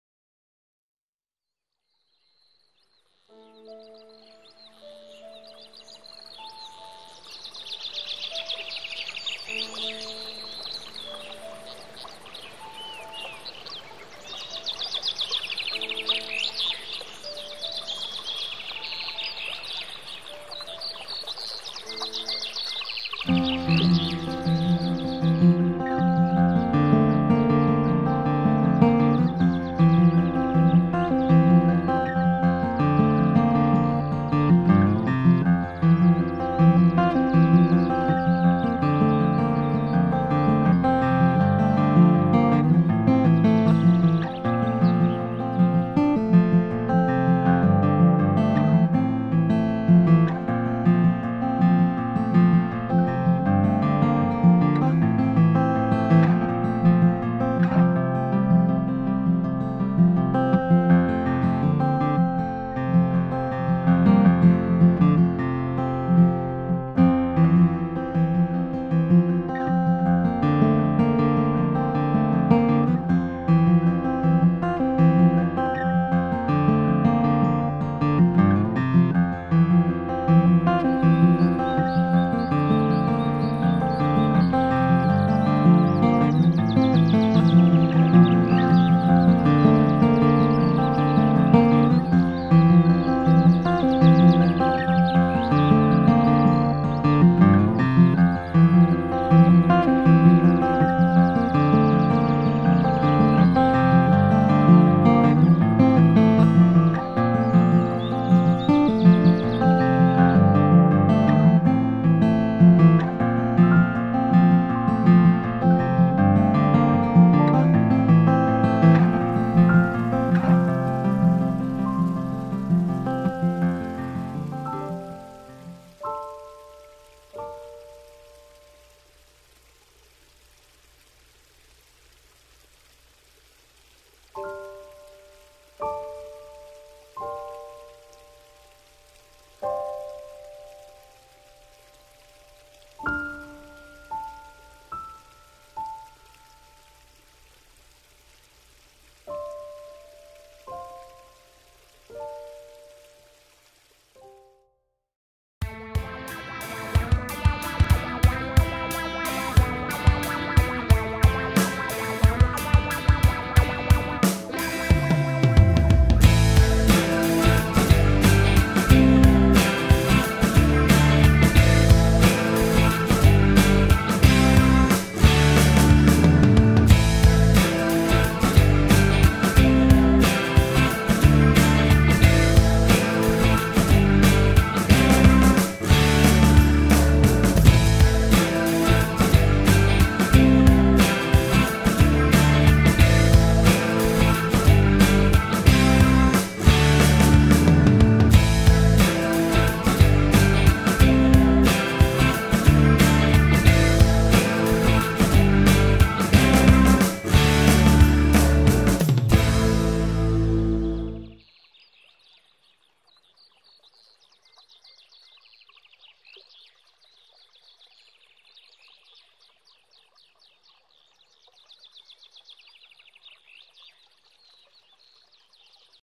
Centro de vacaciones Benageber
Centro de Vacaciones del embalse de Benagéber  , una muestra de las diversas actividades que pueden realizarse.